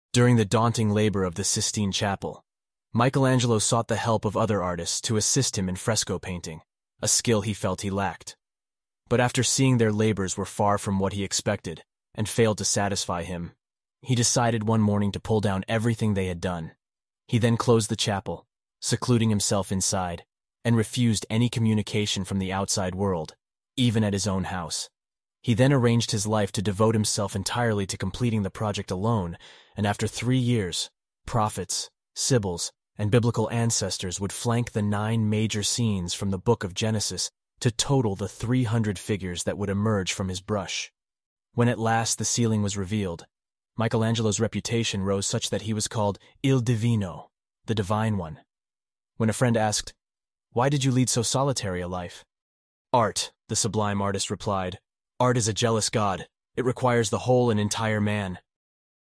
Solitude is the school of genius
During the daunting labor of the Sistine Chapel, Michelangelo sought the help of other artists to assist him in fresco painting, a skill he felt he lacked. But after seeing their labors were far from what he expected and failed to satisfy him, he decided one morning to pull down everything they had done. He then closed the chapel, secluding himself inside, and refused any communication from the outside world, even at his own house. He then arranged his life to devote himself entirely to completing the project alone, and after three years, Prophets, Sibyls, and Biblical Ancestors would flank the nine major scenes from the Book of Genesis to total the 300 figures that would emerge from his brush. When at last the ceiling was revealed, Michelangelo's reputation rose such that he was called il divino, the divine one. When a friend asked, “Why did you lead so solitary a life?”
“Art,” the sublime artist replied, “Art is a jealous God. It requires the whole and entire man.”
'Solitude is the school of genius,' is a quote from the Edward Gibbon.
What contingencies provoke the artistic type of person, possessed by an inner genius, with a mind for creation? The English historian Edward Gibbon has suggested a greater access to our inner source.

"Conversation enriches the understanding, but solitude is the school of genius; and the uniformity of a work denotes the hand of a solitary artist."
--- Decline and Fall. v. 359 – 60.

Once we are relieved from society and refuged in solitude, the mind quiets down to receive our genius. By solemnly cultivating the life of the mind we can achieve a greater encounter with the frequency of our own genius, as Epictetus informs us:

"When you have shut your doors, and darkened your room, remember never to say that you are alone, but God is within, and your genius is within, --- and what need have they of light to see what you are doing?"
--- Discourses, bk i, ch. xiv.

This gave the English poet Walter Savage Landor the following line:
"A solitude is the audience-chamber of God."
--- W. S. Landor, Imaginary Conversations: Lord Brooke and Sir Phillip Sidney.